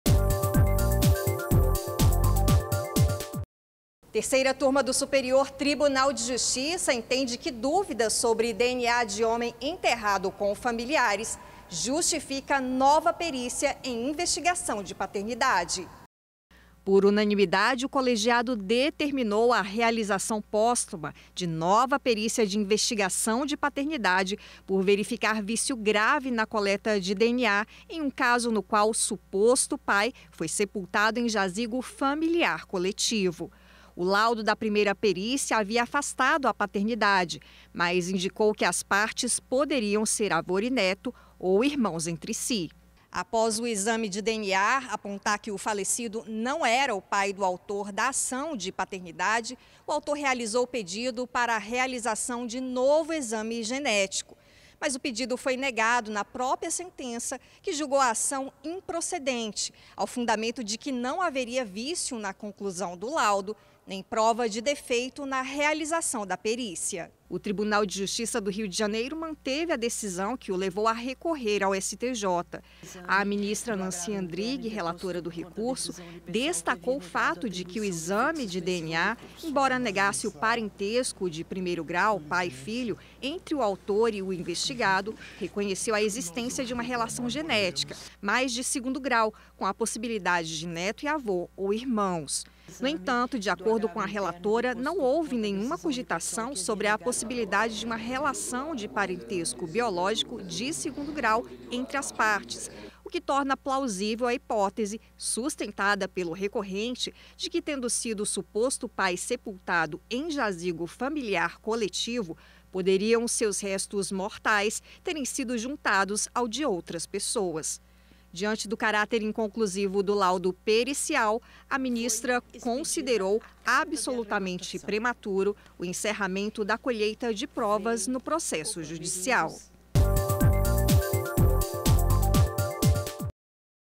Jazigo familiar coletivo gera dúvida em exame de DNA
​Por verificar vício grave na coleta de DNA, a Terceira Turma do Superior Tribunal de Justiça (STJ), em decisão unânime, determinou a realização de nova perícia de investigação de paternidade post mortem em um caso no qual o suposto pai foi sepultado em jazigo familiar coletivo. O laudo da primeira perícia havia afastado a paternidade, mas indicou que as partes poderiam ser avô e neto ou irmãos entre si.
O colegiado considerou plausível a tese de que os restos mortais do suposto pai podem ter se misturado com os de seus familiares, gerando dúvida a respeito do resultado da prova pericial.

O número deste processo não é divulgado em razão de segredo judicial.